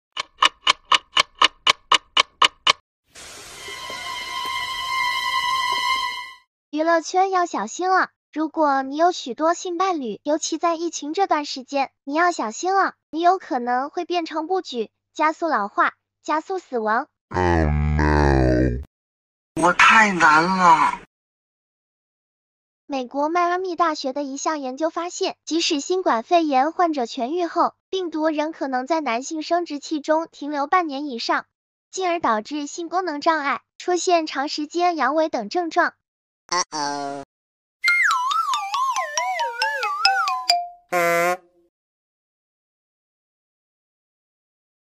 色上头字一把刀，男士们小心不举 😱😱😱
2021/05/21 已经分享了疫情会令男士不举尤其拥有许多性伴侣的男士!
2021/05/21 I have shared that the epidemic will make men impotence, especially men who have many sexual partners!
（迈阿密15日综合电）美国迈阿密大学米勒学院（University of Miami Miller School of Medicine）一项研究发现，即便新冠患者痊愈后，病毒仍可能在男性生殖器中停留半年以上，进而导致性功能障碍，出现长时间阳痿等症状。
(Miami 15th comprehensive report) Miami University Miller College  (University of Miami Miller School of Medicine) A study found that even after patients with COVID-19 are cured, the virus may still stay in the male genitals for more than half a year, leading to sexual dysfunction and symptoms such as prolonged impotence.